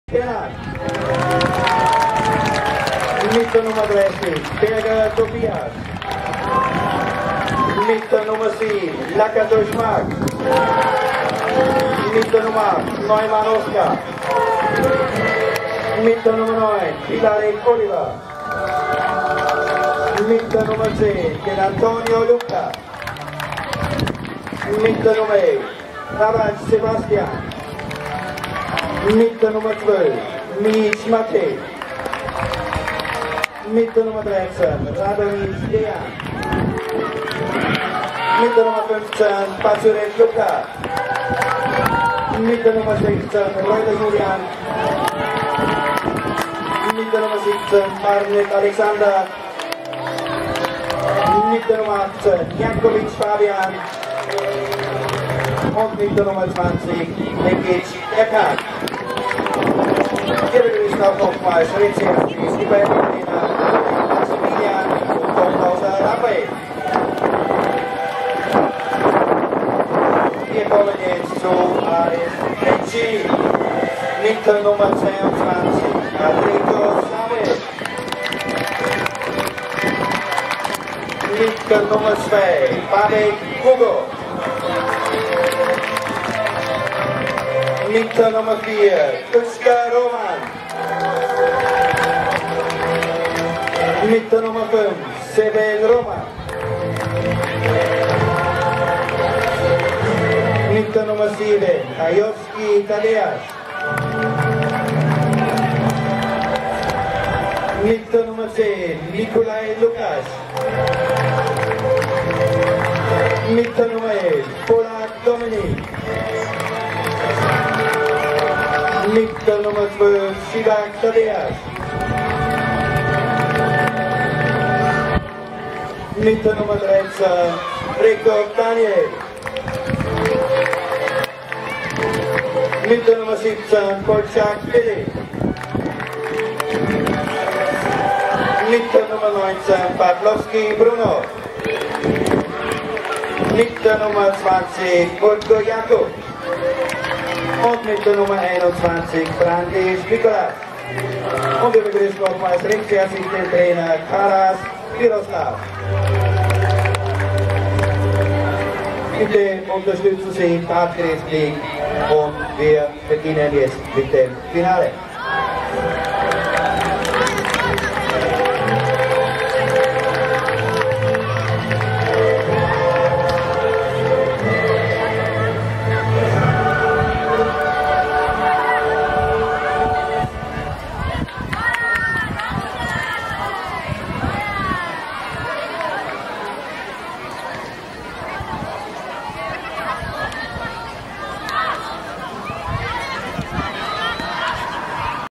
Pannonia Cup 11.06.2017
Interntionales U 12 Tunier in St.Georgen bei Eisenstadt
FK Austria Wien -- FC Veszprem
Mannschaftsaustellung